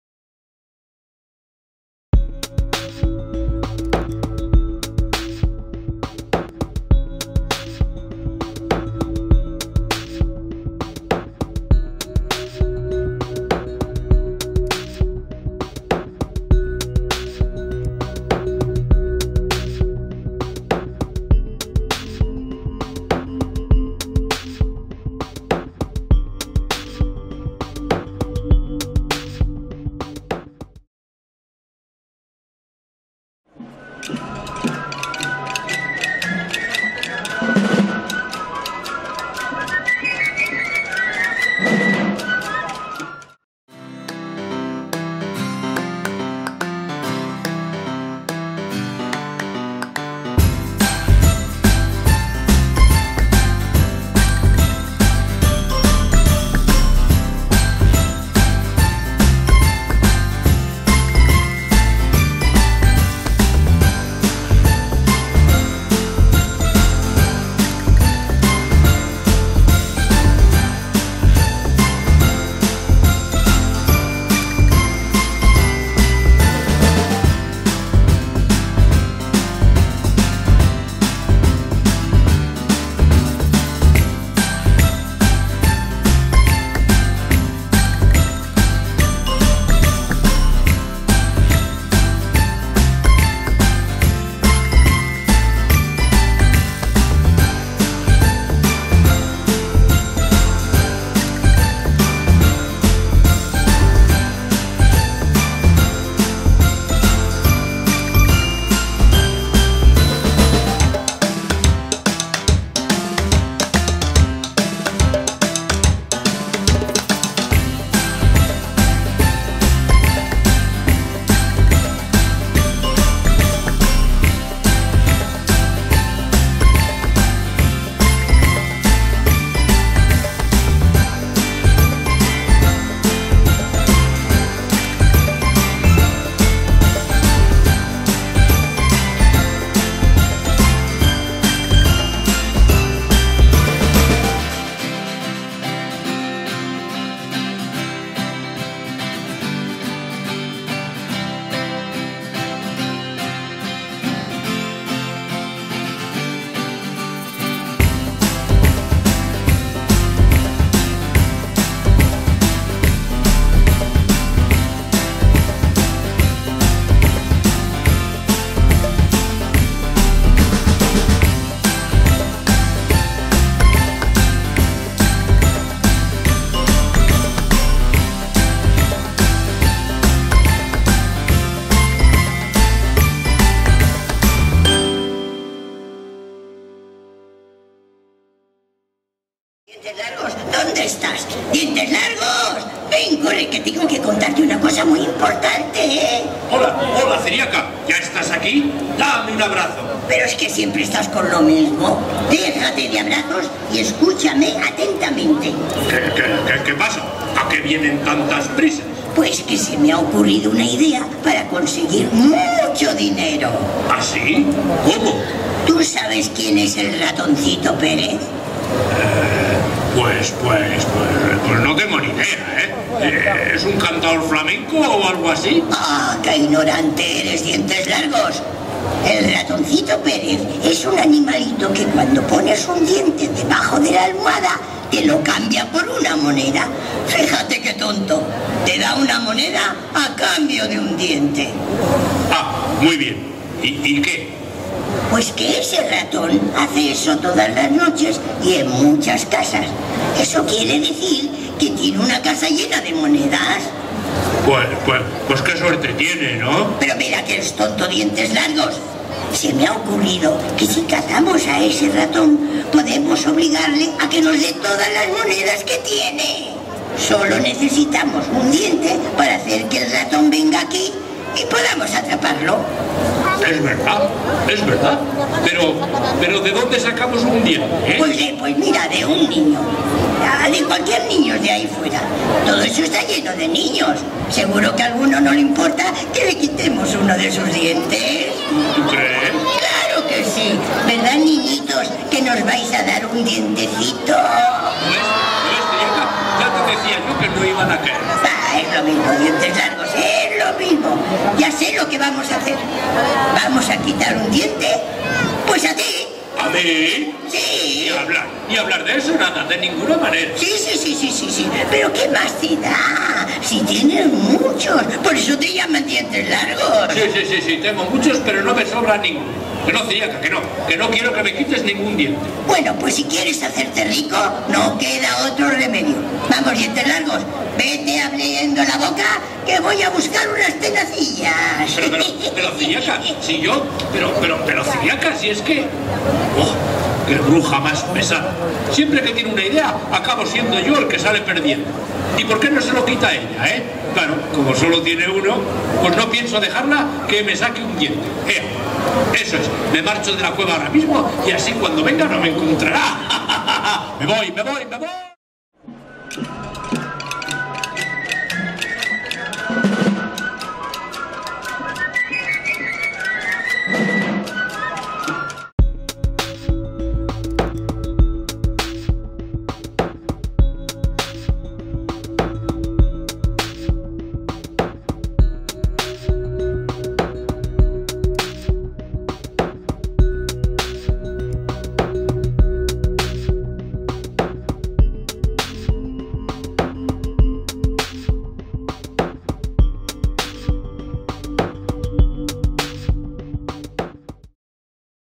Menudas Fiestas - Plaza Conde Rodezno - San Fermín 2015. What a festival. Conde square
¡Menudas Fiestas ! Plaza Conde de Rodezno. Actuaciones y atracciones infantiles. Diversión para los peques en San Fermín, hinchables, juegos, actuaciones...

Marionetas de Maese Villarejo. Plaza Conde de Rodezno. “Gorgorito y el ratoncito Pérez”

What a festival. Conde square